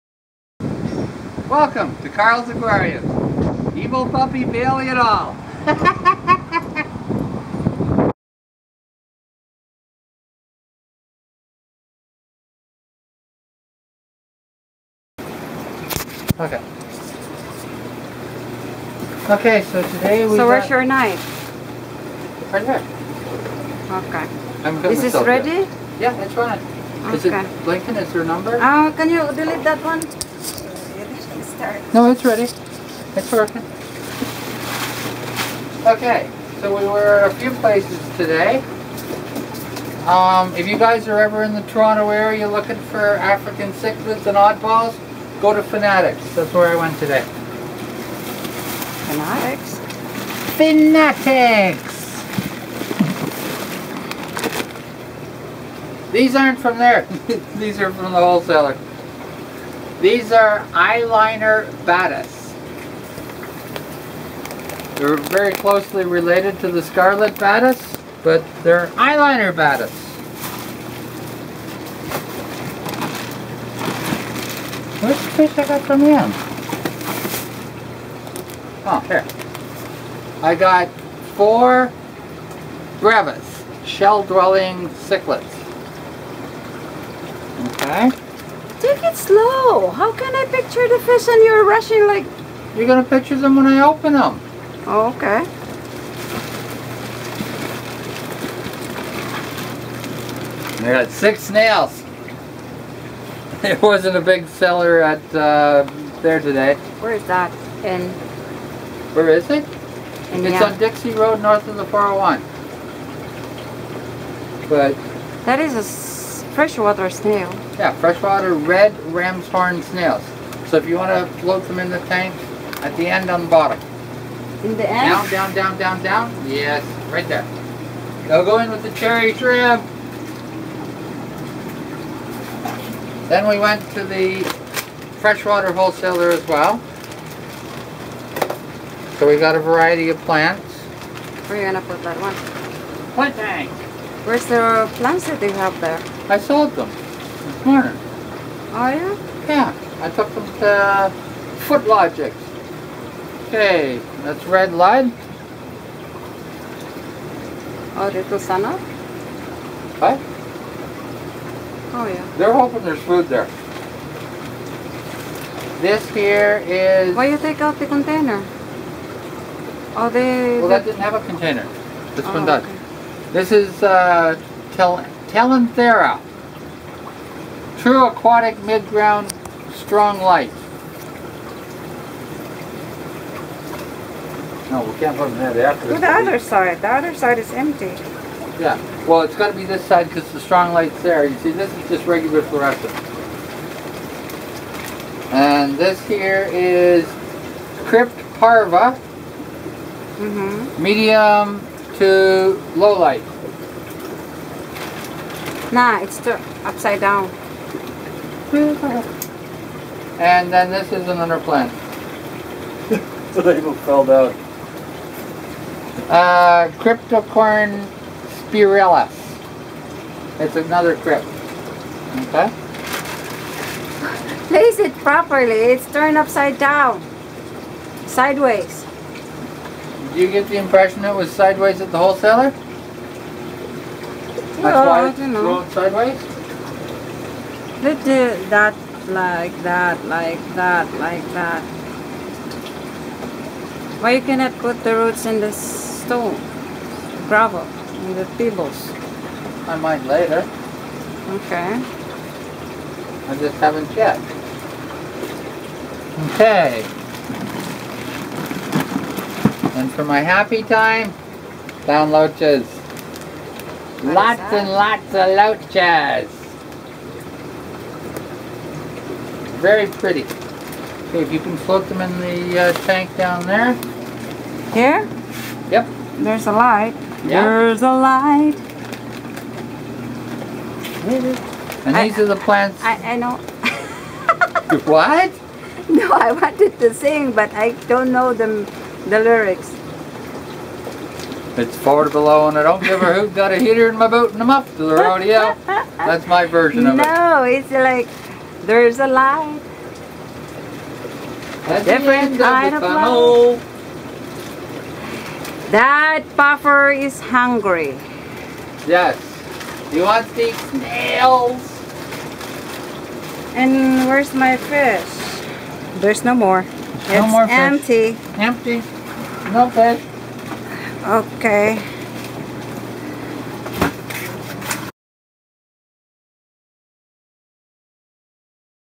#2015 Its A Freshwater Kind Of Day Part One Carls Aquarium
In this video Carl and Fritzie unbox fish from a number of different places!